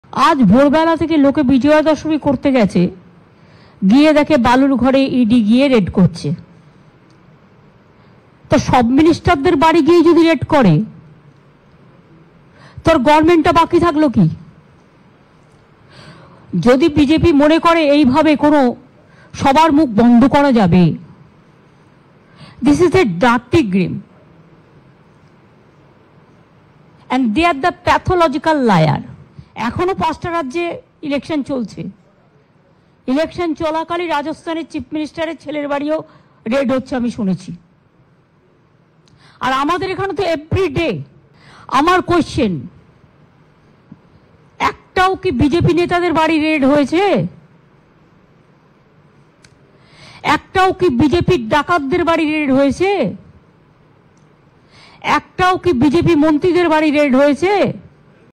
Jyotipriya Mallick র বাড়িতে ED হানা নিয়ে বিস্ফোরক Mamata Banerjee, দেখুন
Mamata Banerjee : Jyotipriya Mallick র বাড়িতে ইডি হানা। এর আগে রথীন-ফিরহাদের বাড়িতেও কেন্দ্রীয় এজেন্সি।  চড়া সুর মুখ্যমন্ত্রীর।Firhad এর বাড়িতে চিনির কৌটো উলটে দেখার মতো বিস্ফোরক অভিযোগ। BJP নেতাদের বাড়িতে কেন তল্লাশি নয়? প্রশ্ন মমতা বন্দ্যোপাধ্যায়ের।

n180c_kolkata | n18_shorts